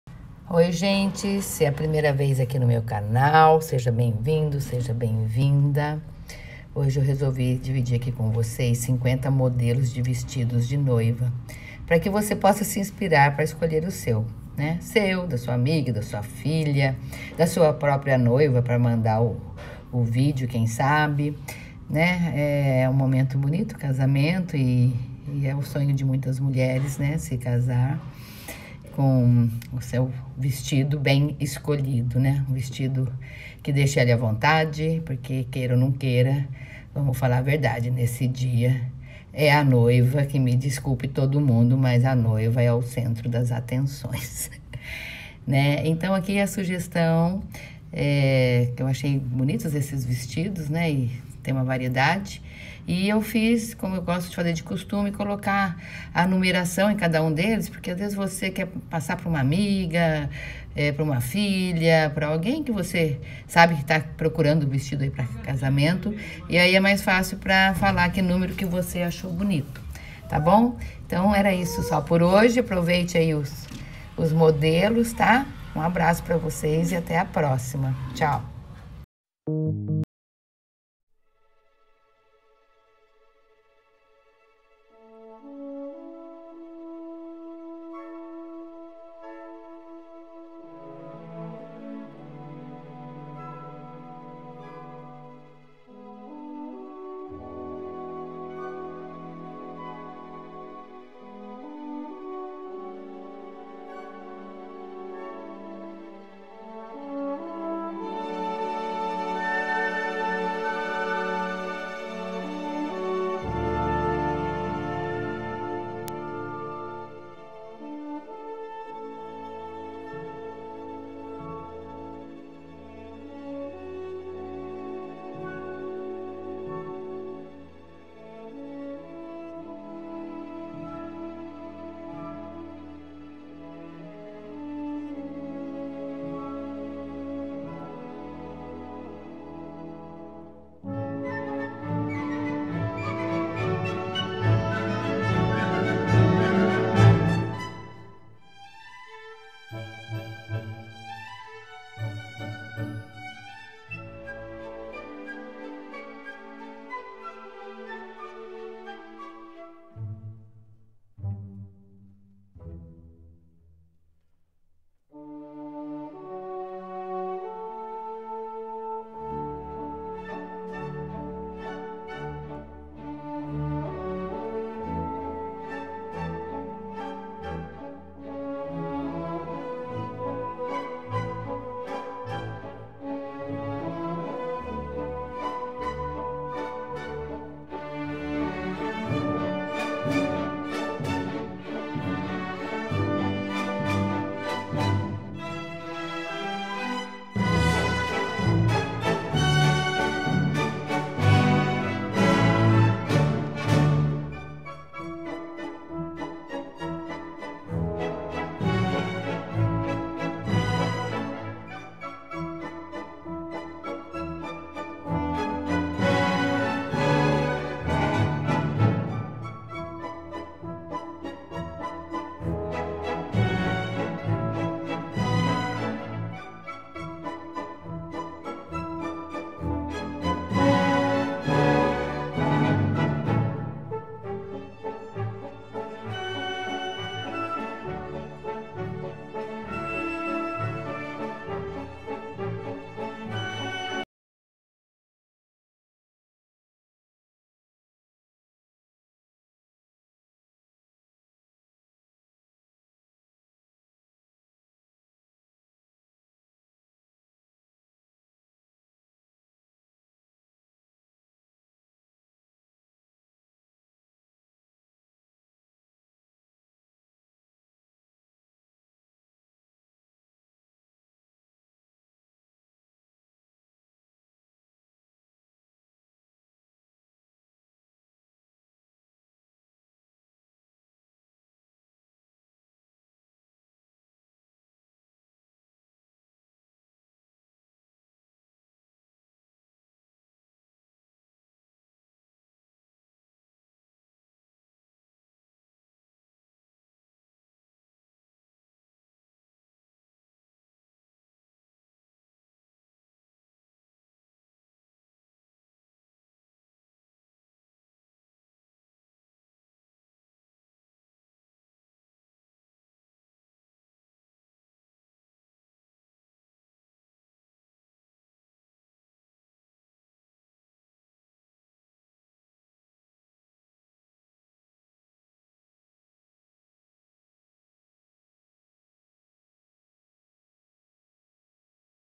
50 modelos de vestidos de noiva para que você possa se inspirar para escolher o seu
50 modelos de vestidos de noiva para você se inspirar e escolher o seu. O video apresenta 50 modelos com
Numeração para que você possa também enviar para uma
Amiga, costureira ou até mesmo para o
Lugar aonde possa estar
Pensando em adquirir o seu.
É interessante antes de ir a procura do seu vestido de noiva que já possa ter uma ideia daquilo que realmente queira, para não sofrer tanta influência doa outros. É um dia especial e não abra mão de bancar o seu desejo.
Um abraço.